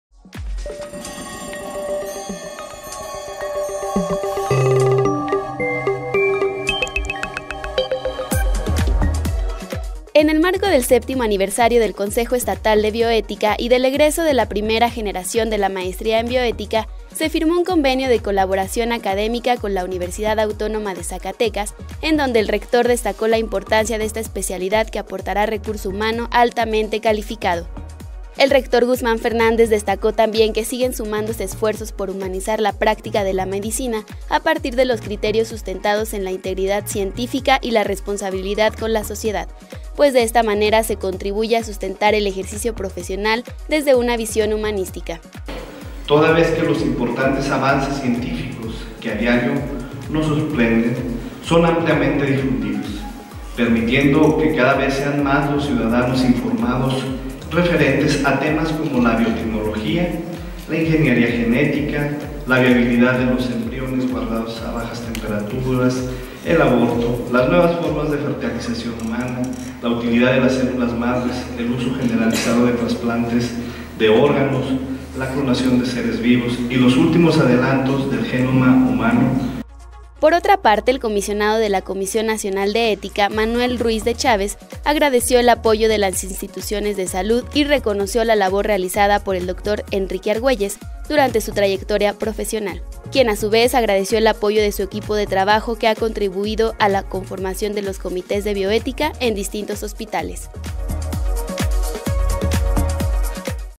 Firma la UAZ convenio de colaboración con el Comité Estatal de Bioética
En el marco del séptimo aniversario del Consejo Estatal de Bioética y del egreso de la primera generación de la maestría en Bioética, se firmó un convenio de colaboración académica con la Universidad Autónoma de Zacatecas, en donde el rector destacó la importancia de esta especialidad que aportará recurso humano altamente calificado.